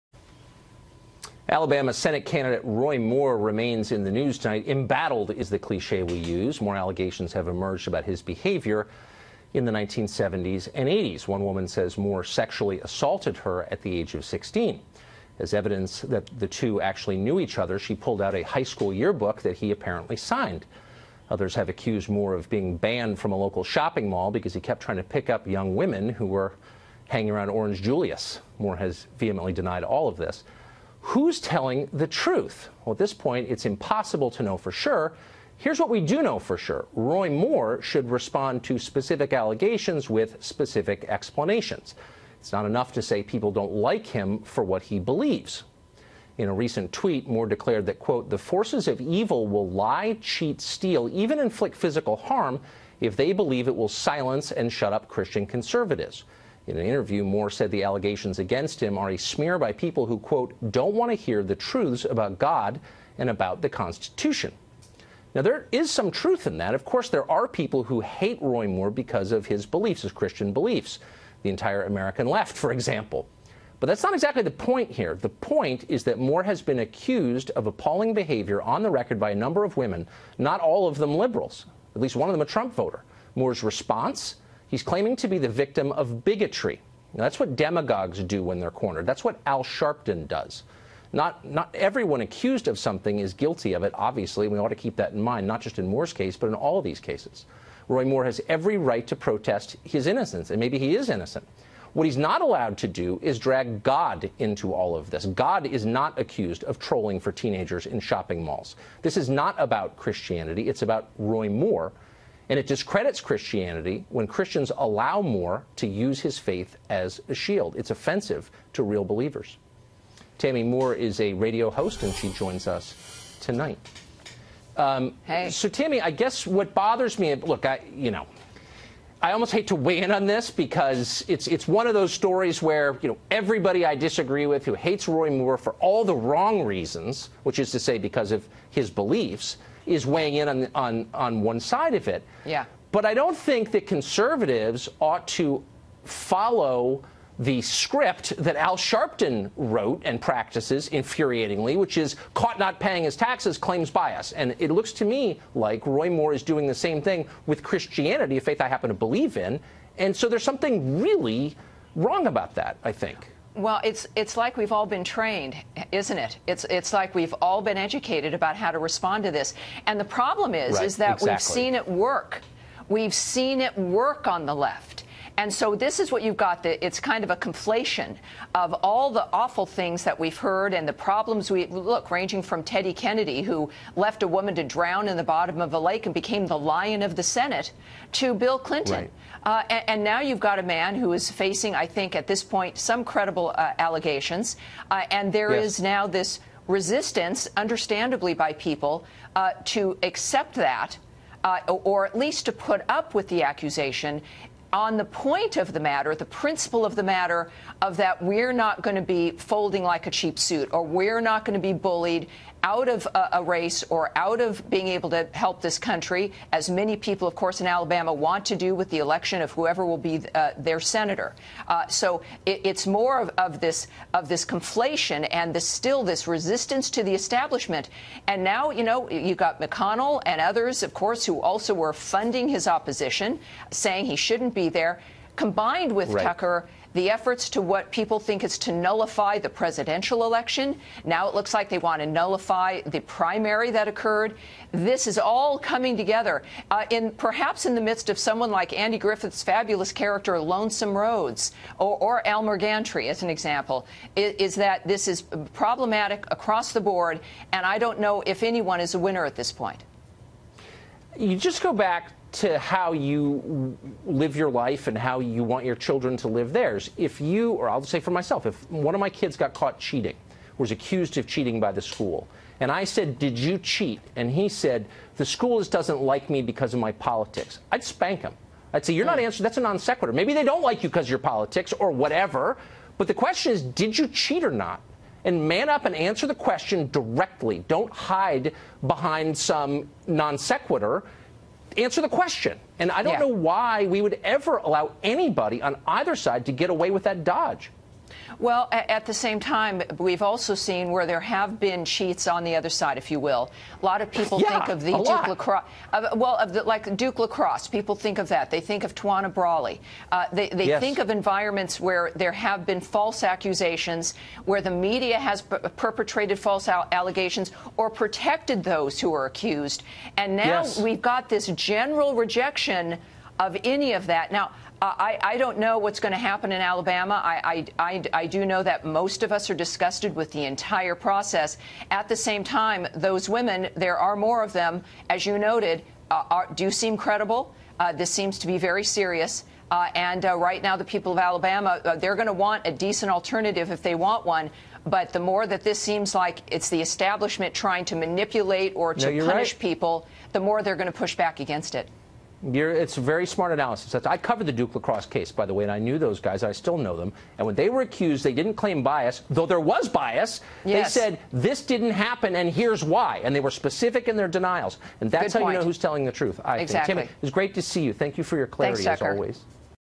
[ Tucker Porn ] Tucker Roy Moore hides behind faith
Tucker Roy Moore hides behind faith